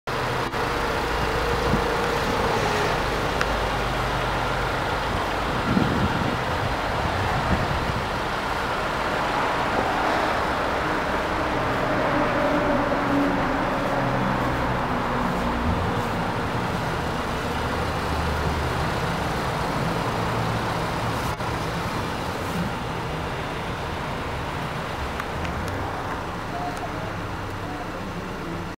2014 Jeep Cherokee Limited | Granite Crystal | EW201304 | Seattle | Bellevue
Ten Most Fun to Drive Cars Award!!  -- Cherokee Limited, 4D Sport Utility, 2.4L I4 MultiAir, 9-Speed Automatic, 4WD, Granite Crystal Metallic Clearcoat, Black/Iceland Gray w/Leather Trimmed Bucket Seats or Premium Leather Trimmed Bucket Seats, ** SATELLITE RADIO**, **ALLOY WHEELS**, **BLUETOOTH**, **HEATED SEATS**, **LEATHER**, **NAVIGATION**, and 9 Amplified Speakers w/Subwoofer.